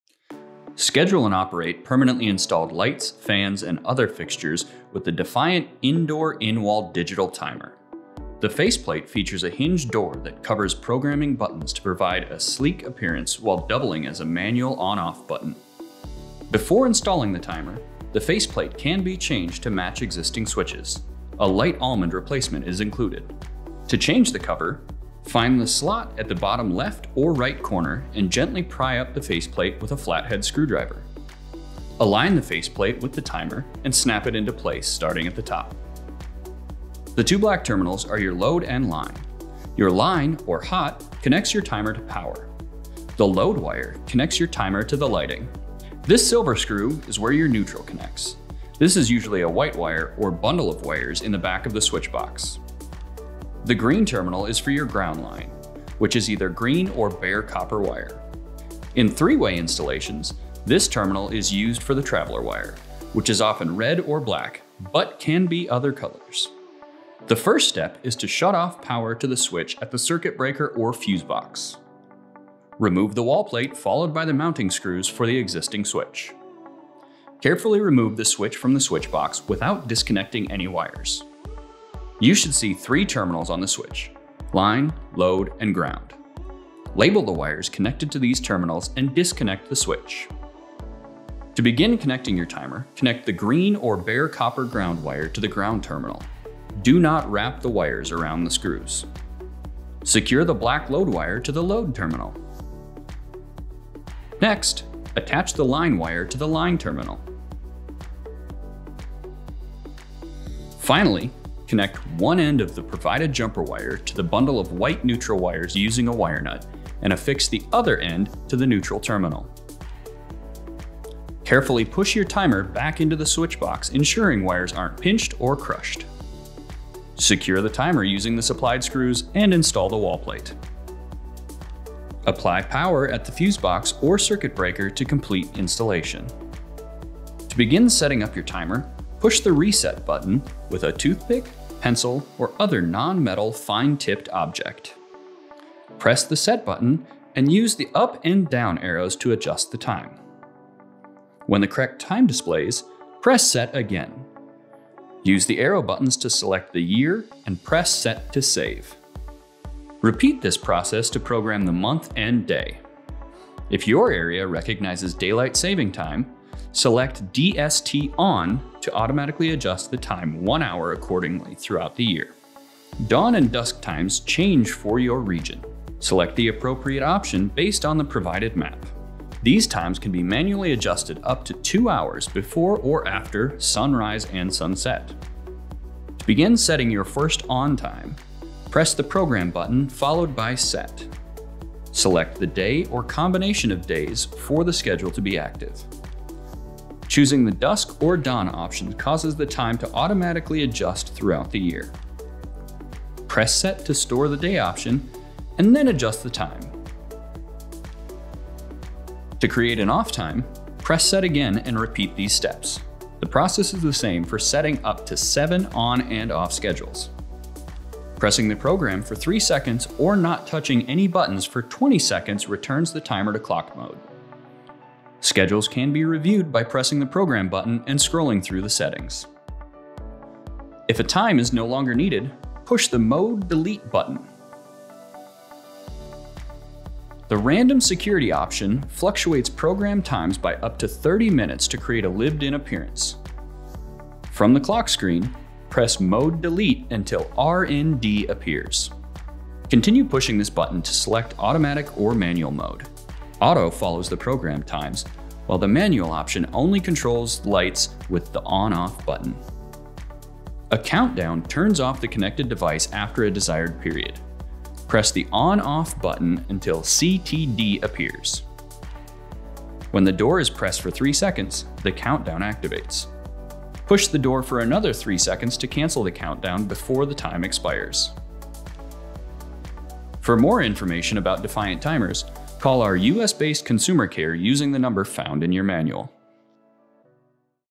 32648: Defiant 7-Day In-Wall Timer - Installation and Setup
Your switch does the work for you with the Defiant 7-Day In-Wall Digital Timer. By supporting up to seven ON/OFF schedules, the timer is ideal for varied routines. A seven-day cycle allows different schedules based on the day of the week. It provides simple, convenient automation for your hardwired lighting in single-pole or three-way installations.

In addition to custom ON/OFF programs, dusk and dawn settings enhance scheduling options. The random feature fluctuates programmed times by up to 30 minutes to create a lived-in appearance. A countdown function automatically turns off lights after a desired period. Built-in self-adjusting technology automatically recognizes the sunrise and sunset times for your region and makes the appropriate adjustment to your lighting settings.